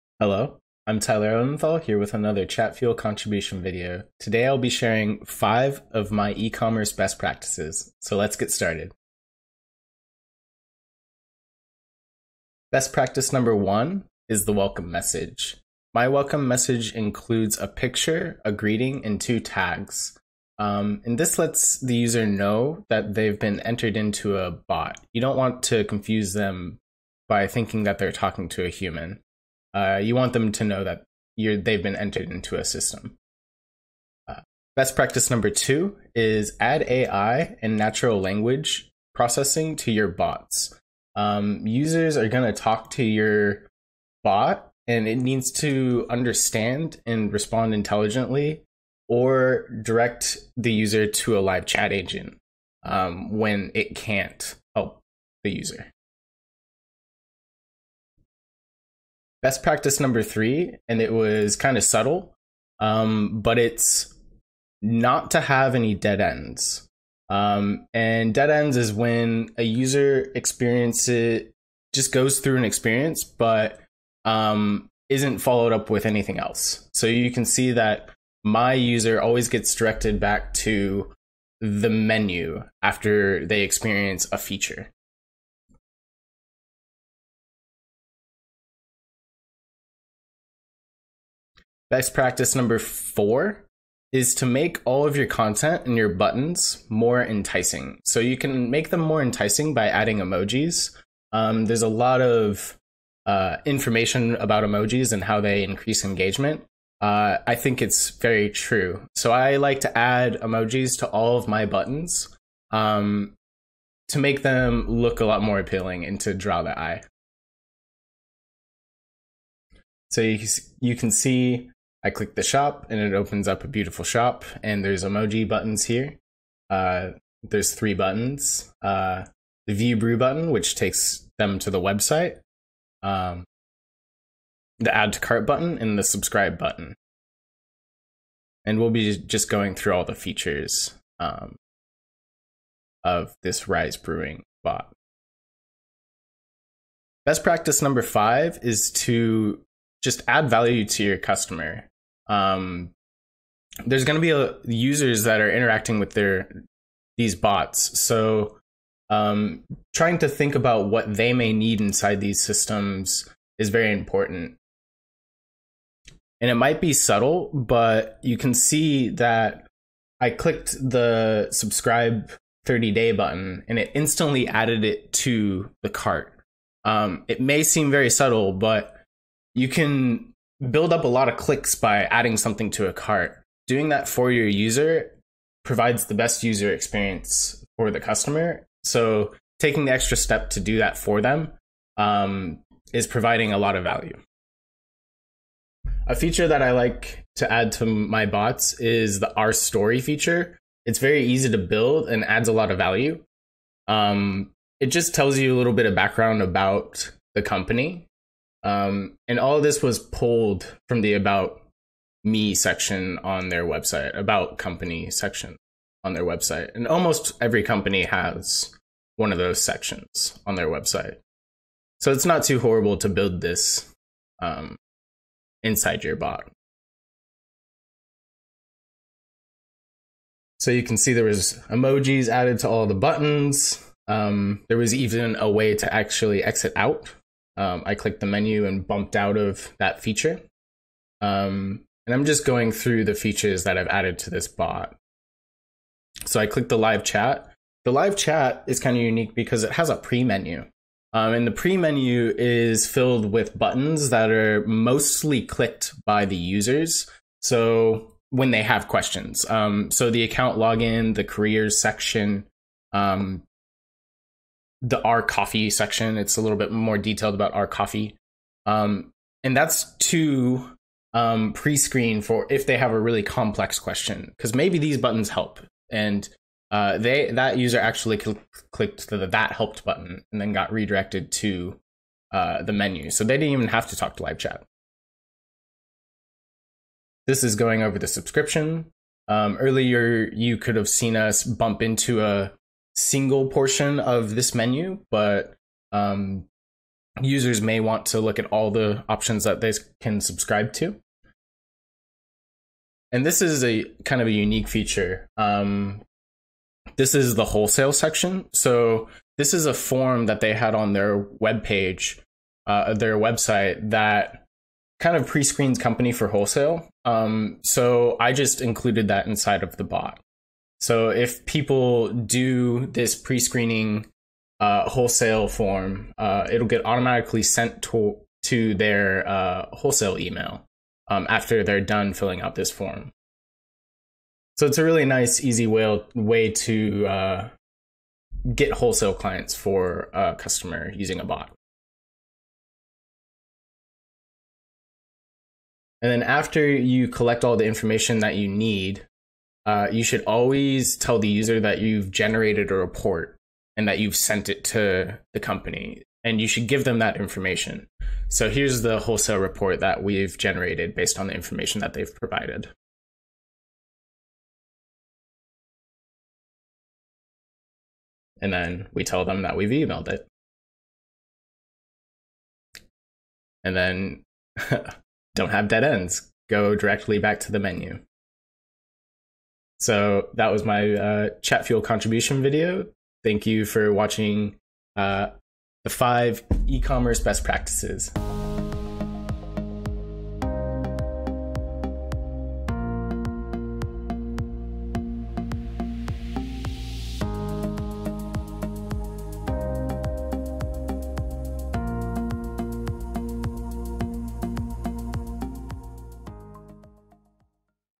5 Ecommerce Marketing Tips for Facebook Messenger Chatbots
Your Facebook Messenger bot is the gateway to boosting sales for your ecommerce business. Contributor Tyler Odenthal shares his favorite tips and tricks for building ecommerce chatbots.

#1: Set subscriber expectations by informing them that they are chatting with a Messenger bot, not a human.

#2: Integrate NLP (Natural Language Processing) to your chatbot since users will inevitably type their own messages to your bot, even if you tell them to stick to the buttons and menus.

#3: Include menu navigation quick replies at the end of each block to avoid "dead ends" in the chat experience, causing drop off.

#4: Spice up your buttons and quick replies with emojis to make them more enticing and visually appealing so that users will click through.

#5: Add value to your subscribers by going the extra mile and ensuring the chat experience saves subscribers time, money, or both.

🌟 P.S. Learn more about why your ecommerce business NEEDS a chatbot